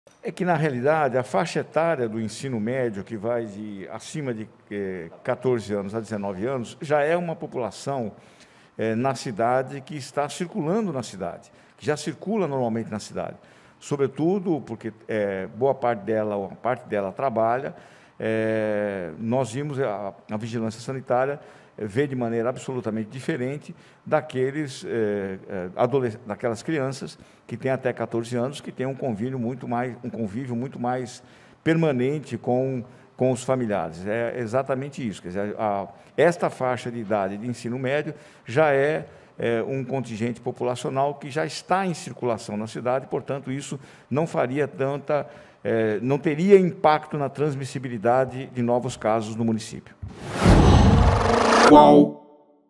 Secretário diz que alunos do ensino médio foram autorizados a voltar porque já circulam por SP
O secretário municipal de Saúde de São Paulo, Edson Aparecido dos Santos, justificou hoje a volta das aulas regulares apenas no ensino médio na capital com o argumento de que esses estudantes já estão circulando em grande parte pela cidade.

Os alunos dessas séries poderão voltar de forma voluntários às escolas públicas e privadas da cidade a partir de 3 de novembro.